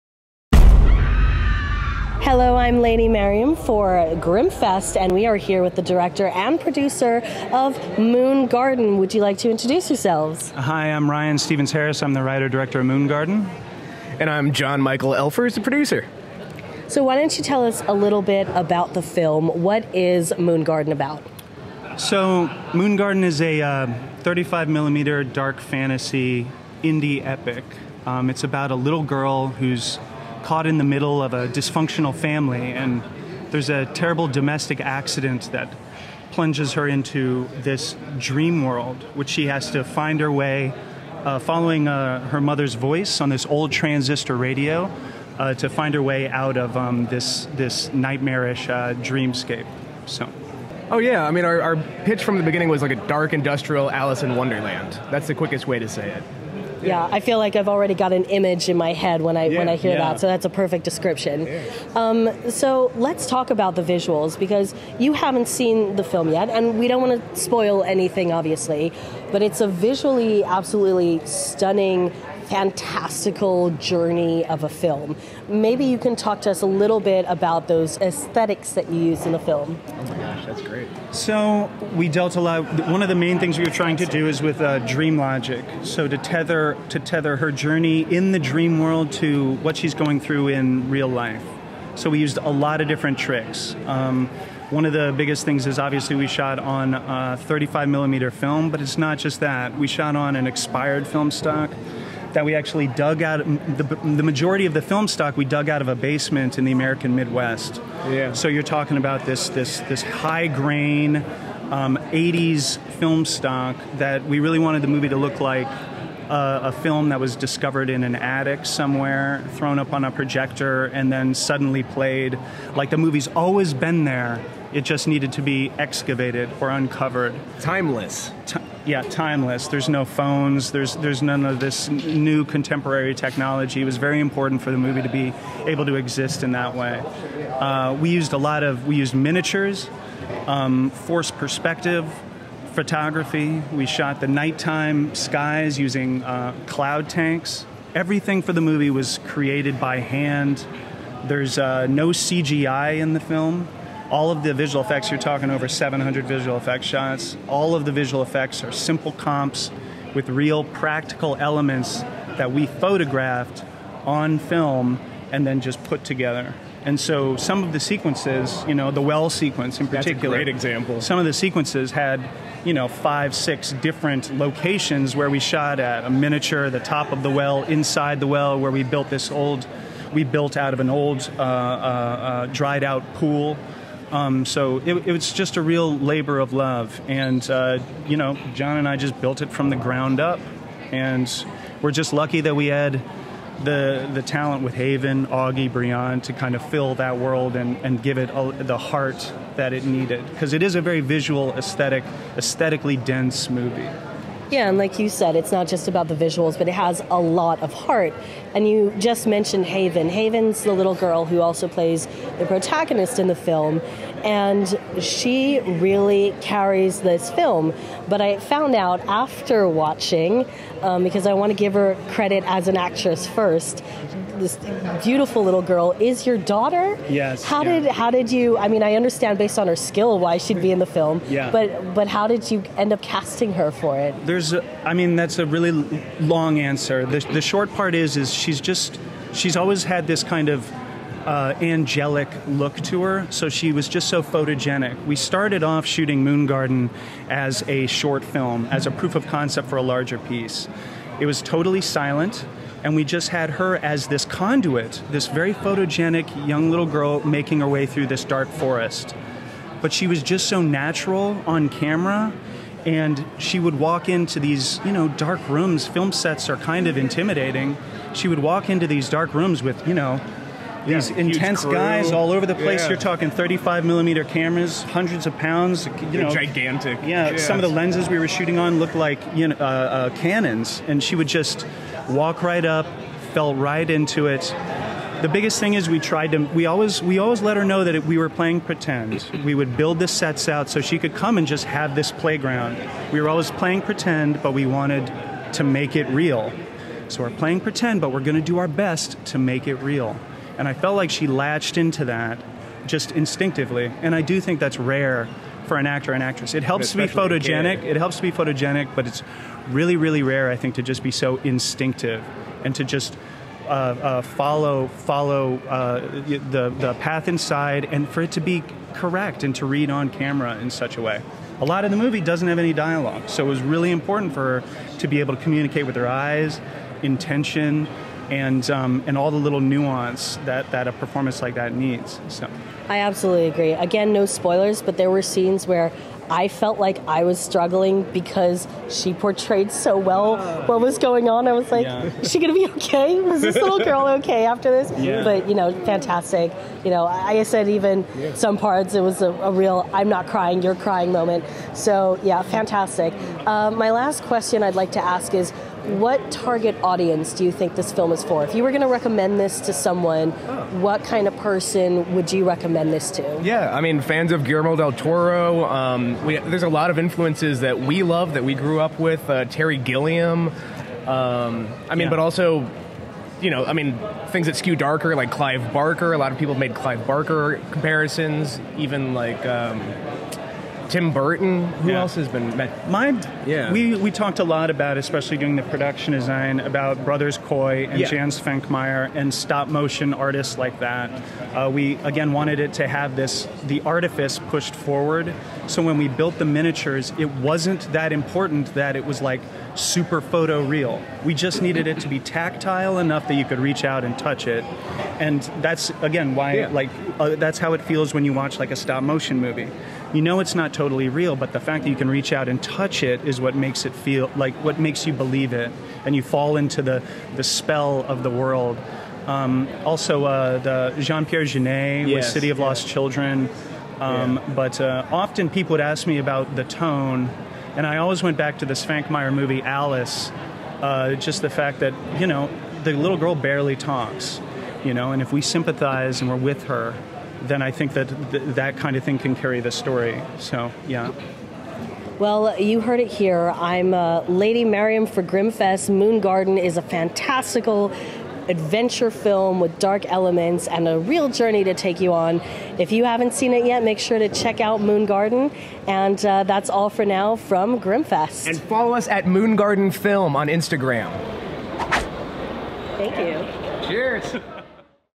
Moon Garden [Official 2022] Interview | Media Wall
Ryan Harris and John Michael Elfers, the director and producer of MOON GARDEN, join the Grimmfest press team at the Media Wall at Grimmfest 2022.

Special thanks to Lady Mariam and Headcinema Productions.